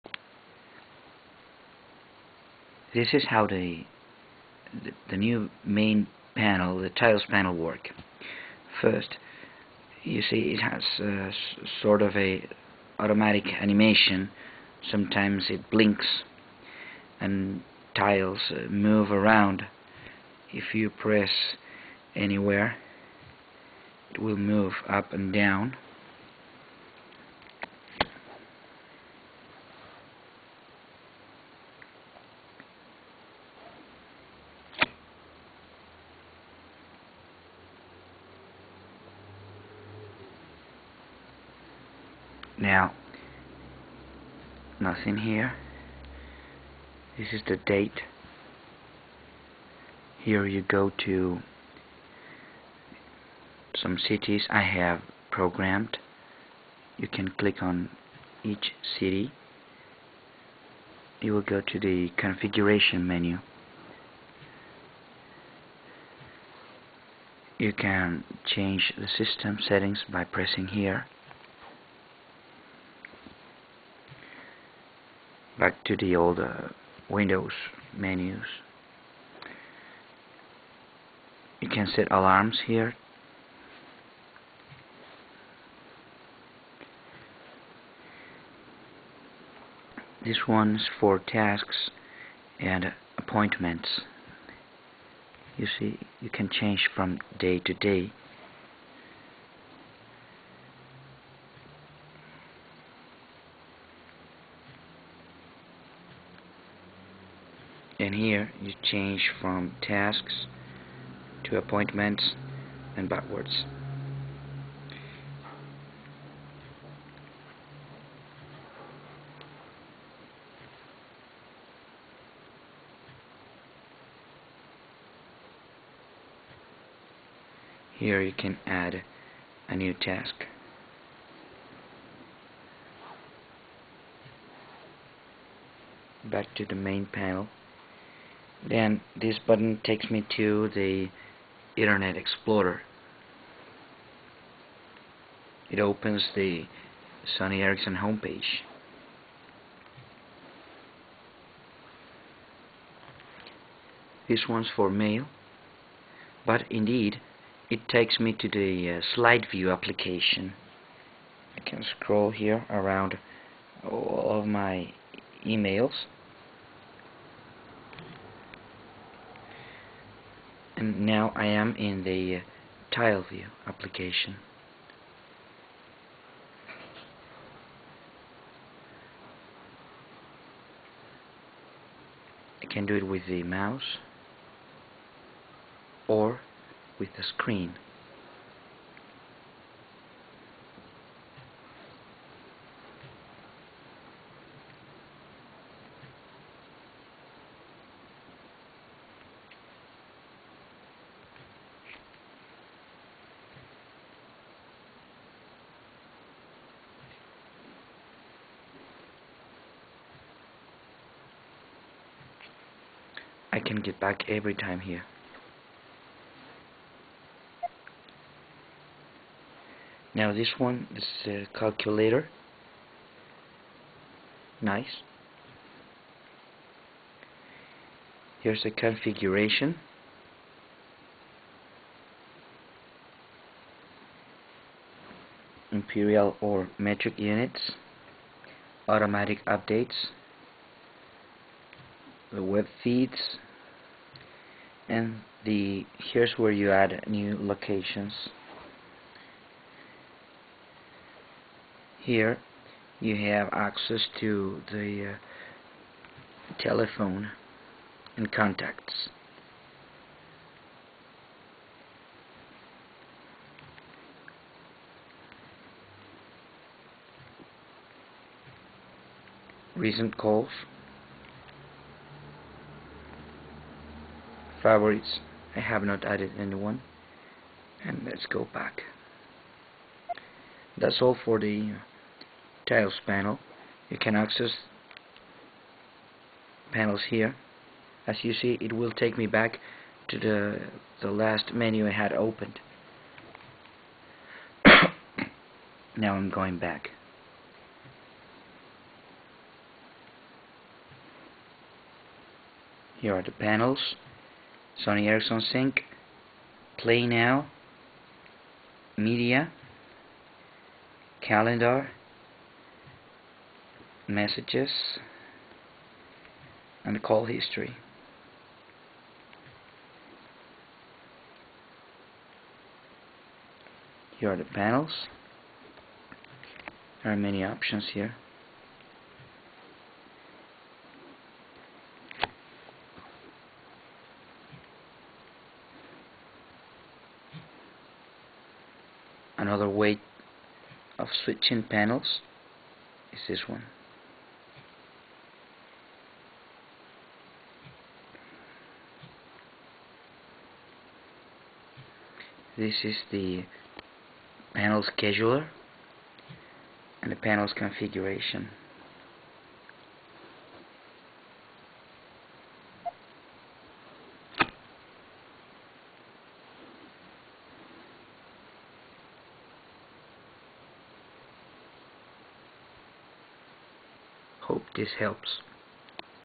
1/2 - XPERIA X2 - Exploring New SlideView UI & Tiles Panel
Part 1 of 2: Exploring the New "SlideView" UI in the XPERIA X2: Panel Manager button has been replaced for "SlideView" button, and the panel manager itself has been integrated into SlideView. Tiles Panel is the most SlideView-integrated panel and is very useful and cool.